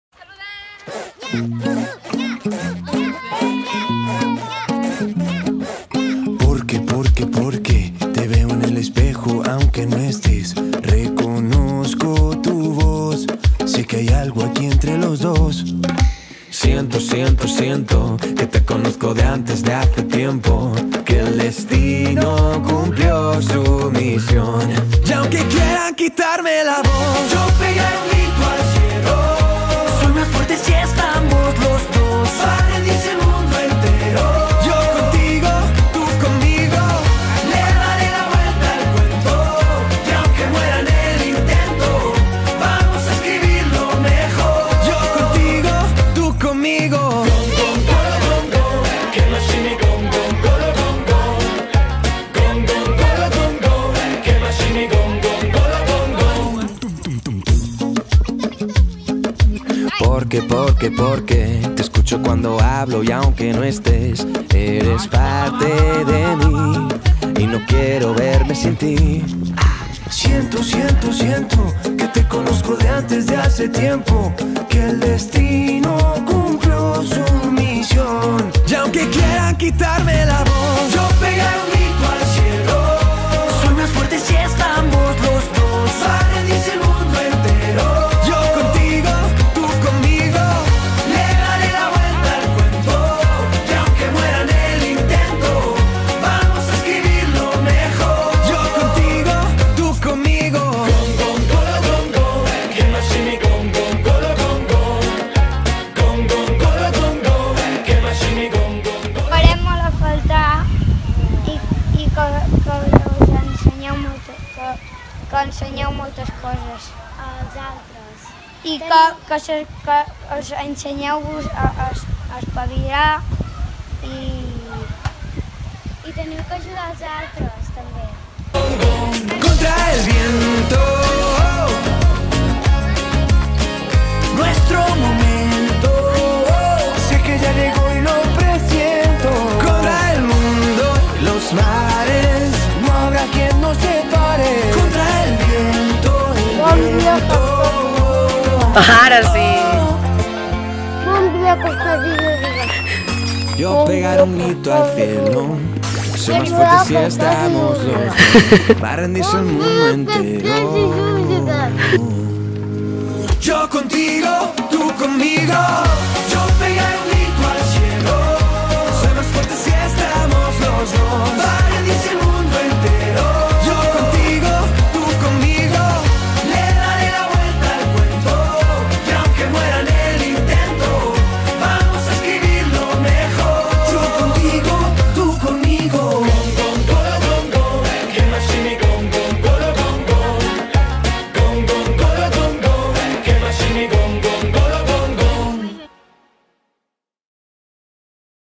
[CiLl's] Sortida d'estiu 2017
Sortida d'estiu dels Castors i Llúdrigues a La Bisbal d'Empordà